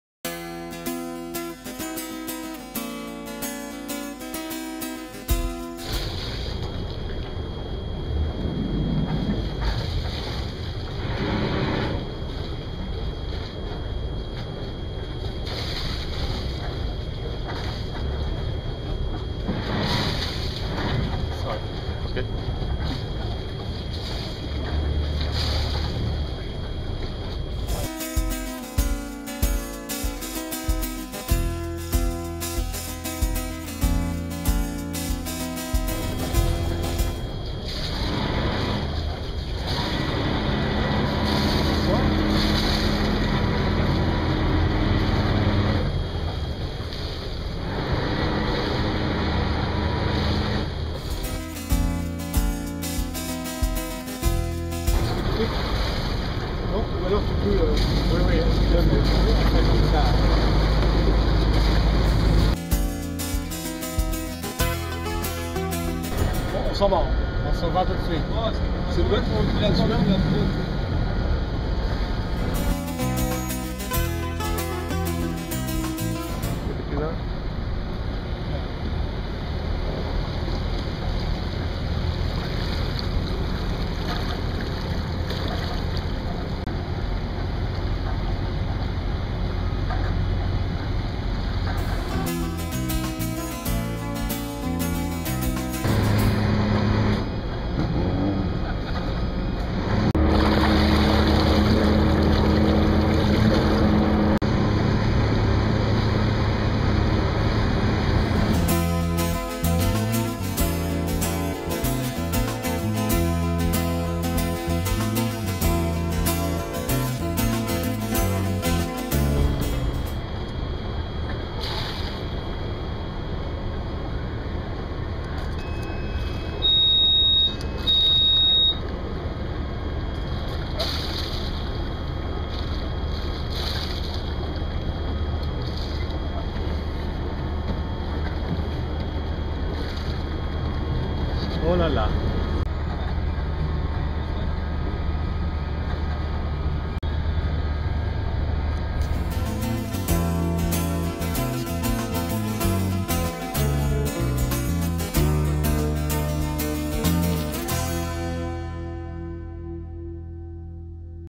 360 Docking Tested by Yachting Monthly
Yachting Monthly tests Jeanneau's revolutionary new 360 Docking system